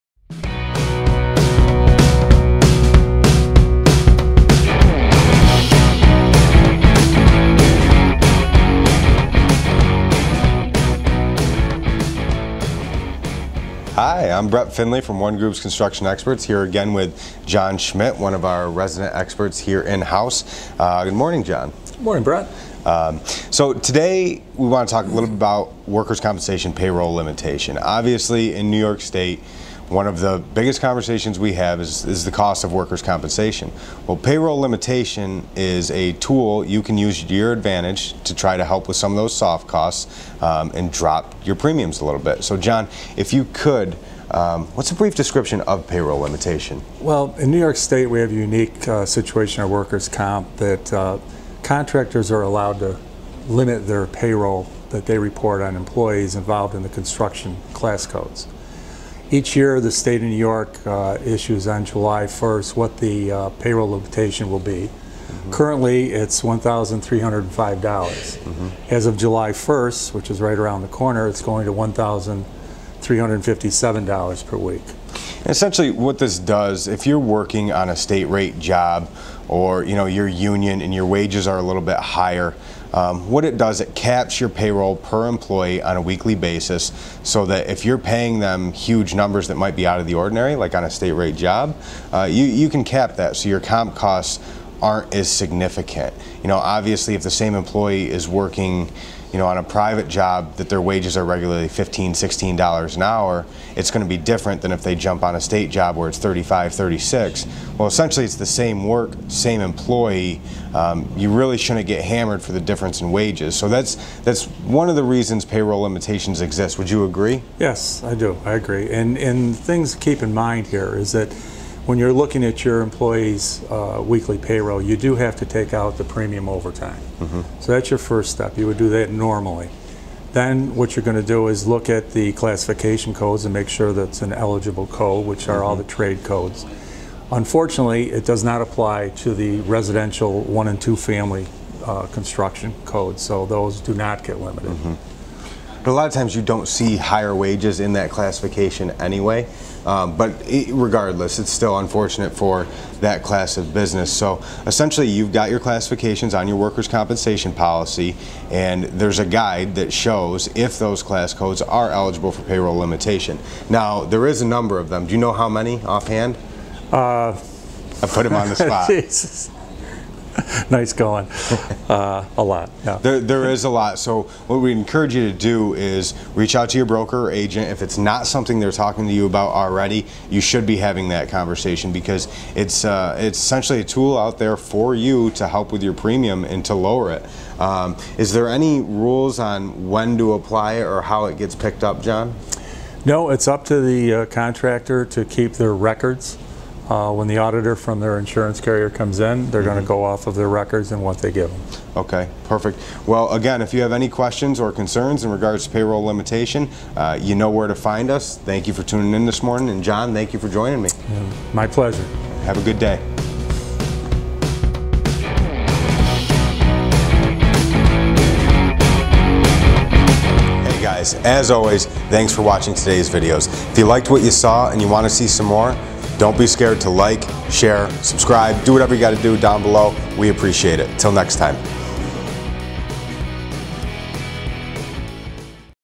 Ep. 2.6 - Workers' Compensation Payroll Limitation
For questions or comments, feel free to visit The Construction Experts

In episode six, John Schmitt explains some of the ins and outs of classifying your payroll for workers' compensation.